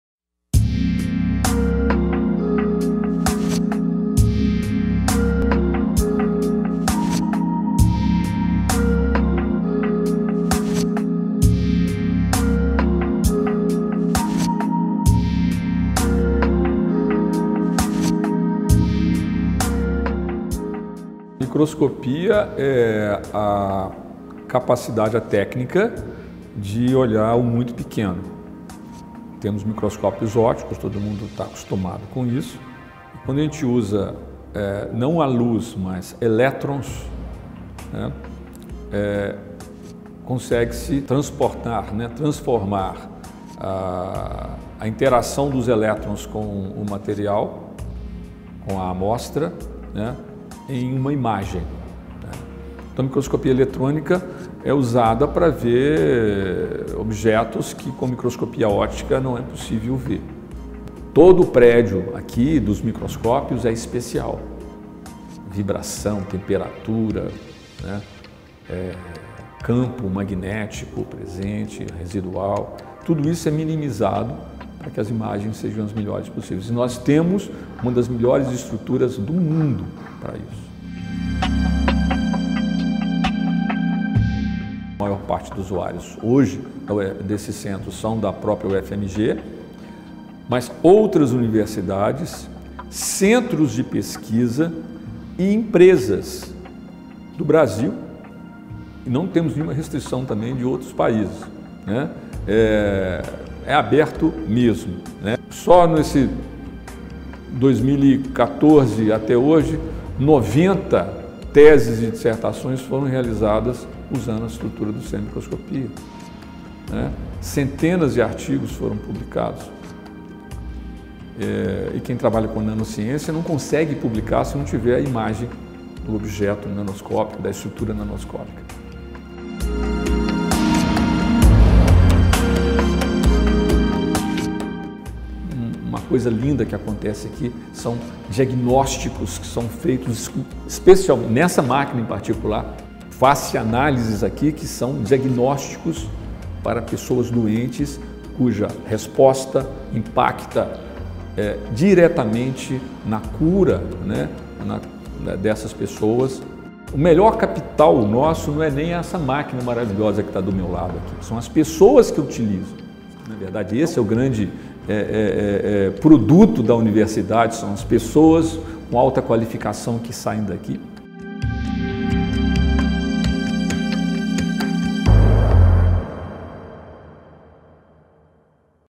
Centro de Microscopia da UFMG abre as portas para o público no aniversário de 10 anos
Data de exibição: 17/11
Sinopse: Inaugurado em 2006, o Centro de Microscopia da UFMG proporciona, aos seus usuários, infraestrutura em técnicas de microscopia com padrão de excelência internacional. Para marcar o aniversário de 10 anos, o centro abriu as portas de seus laboratórios para visitas monitoradas.
Entrevistado: Wagner Rodrigues (diretor Centro de Microscopia UFMG)
Produção e reportagem: Alice Machado
Equipe técnica: Cássio de Jesus
Edição de Imagens: Paulo Guerra